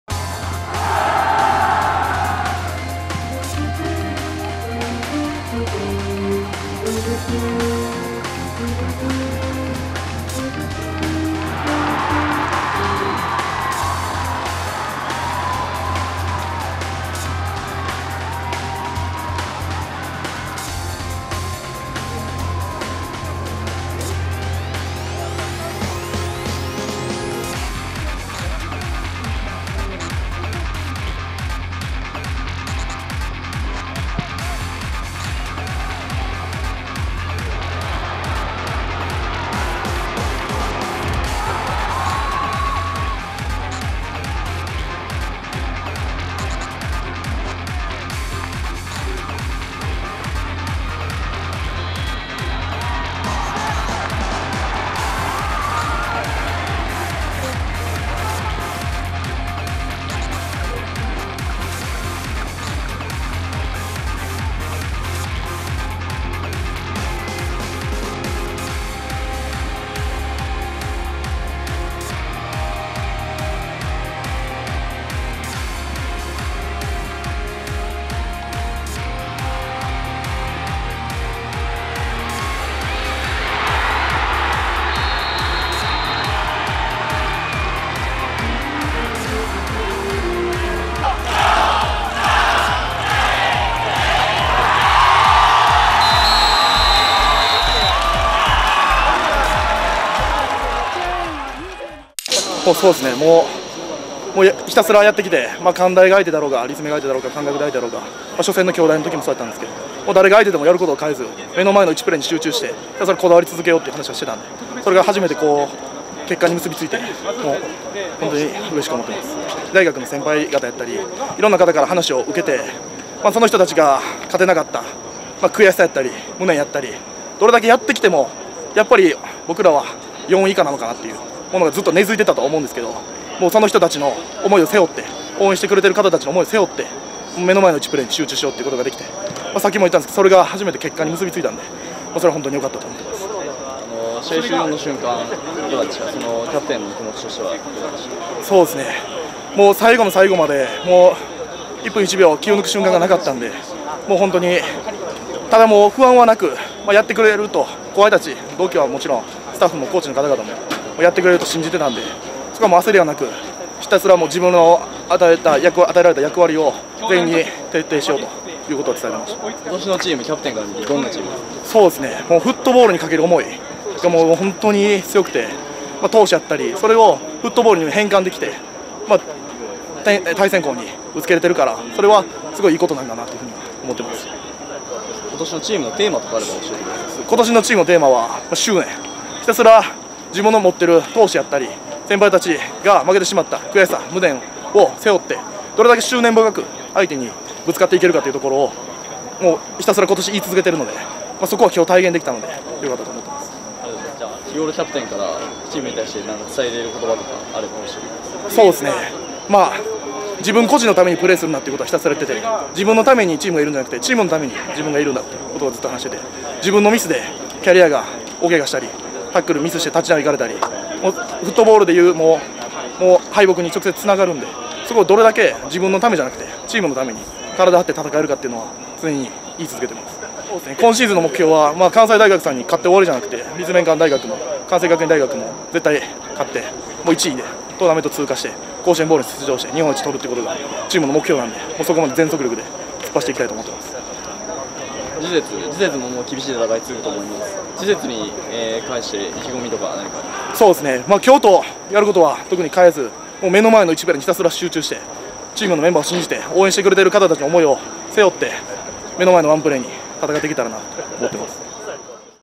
★【関西学生アメフト】歴史的勝利！ 近畿大学・主将 大西勇樹 第２節試合後インタビュー！ 【あすリートチャンネル】
＊１００文字以内にまとめる。

あすリート #@@@（競技名） #@@@@（大会名）

(C)ytv